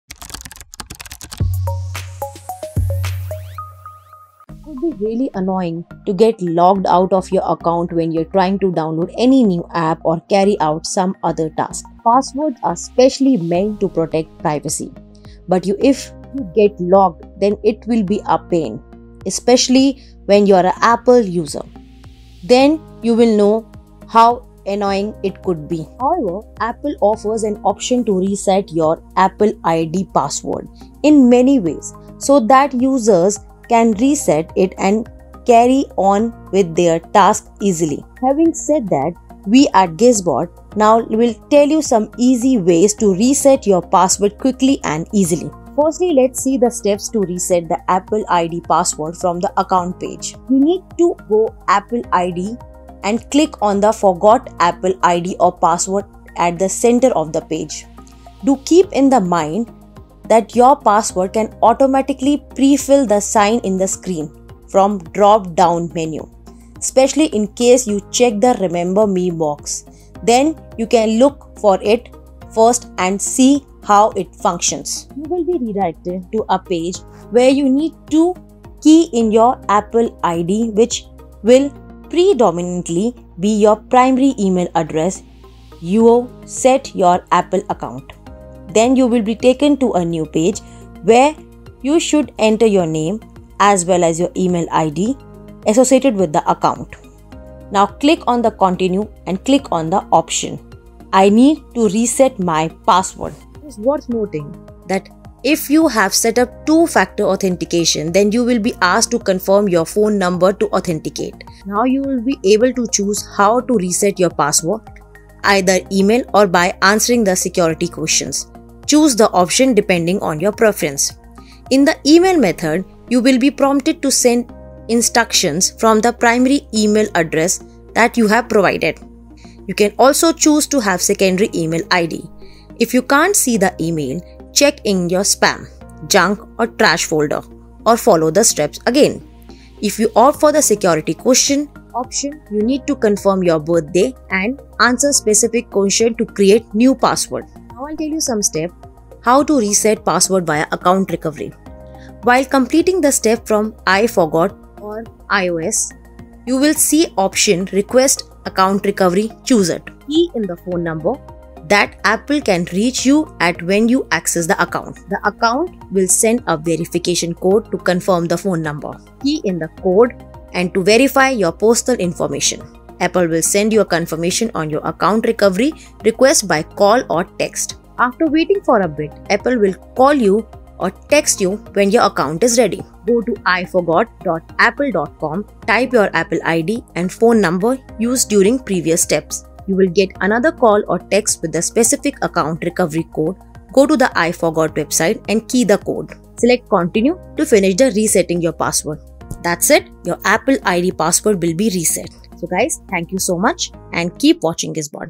How To Reset Your Apple ID Password
It could be really annoying to get locked out of your account when you are trying to download any new app or carry out some other task. Passwords are meant to protect privacy but if you get locked, then it will be a pain. Especially, if you are an Apple user, then you will know how annoying it could be